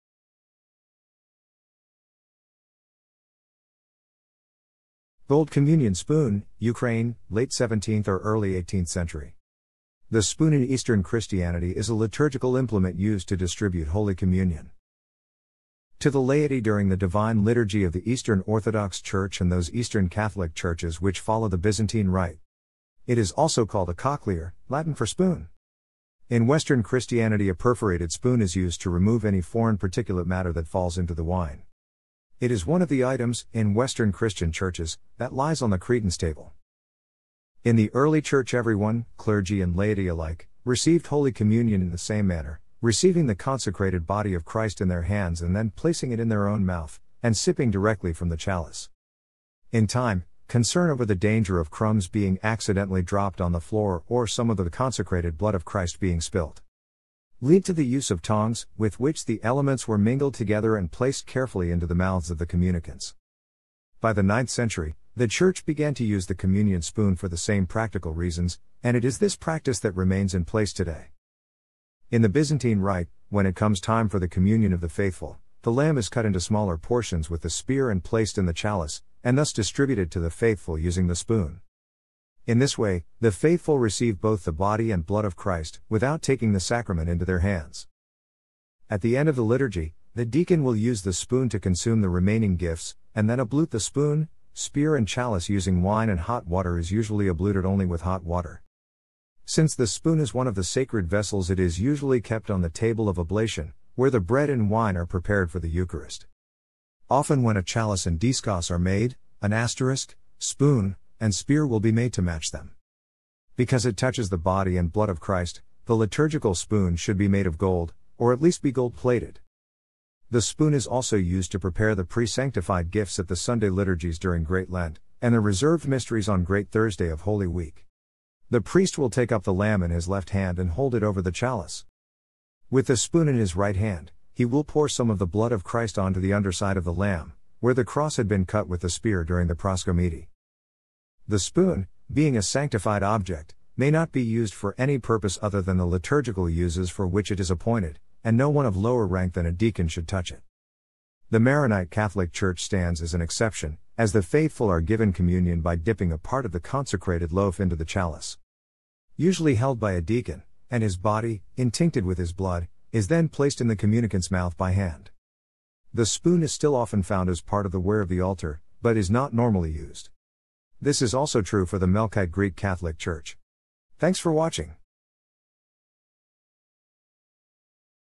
Spoon (liturgy)
Gold communion spoon, Ukraine, late 17th or early 18th century （State Historical Museum, Moscow）.
The Spoon （Greek: Κοχλιάριον, Kochliárion; Slavonic: Лжица, Lzhítza） in Eastern Christianity is a liturgical implement used to distribute Holy Communion
to the laity during the Divine Liturgy of the Eastern Orthodox Church and those Eastern Catholic Churches which follow the Byzantine Rite.
It is also called a cochlear, Latin for "spoon".
In Western Christianity （within traditions such as Anglicanism, Lutheranism, and Methodism） a perforated spoon is used to remove any foreign particulate matter that falls into the wine.
It is one of the items, in Western Christian Churches, that lies on the credence table.
In the Early Church everyone, clergy and laity alike, received Holy Communion in the same manner: receiving the consecrated Body of Christ （in the form of bread） in their hands and then placing it in their own mouth, and sipping directly from the chalice.
In time, concern over the danger of crumbs being accidentally dropped on the floor or some of the consecrated Blood of Christ （in the form of wine） being spilt,
lead to the use of tongs, with which the elements were mingled together and placed carefully into the mouths of the communicants.
By the 9th century, the Church began to use the Communion spoon for the same practical reasons, and it is this practice that remains in place today （though the clergy still receive in the ancient manner as they stand at the Holy Table）.
In the Byzantine Rite, when it comes time for the Communion of the faithful, the Lamb （Host） is cut into smaller portions with the Spear and placed in the Chalice, and thus distributed to the faithful using the Spoon.
In this way, the faithful receive both the Body and Blood of Christ, without taking the Sacrament into their hands.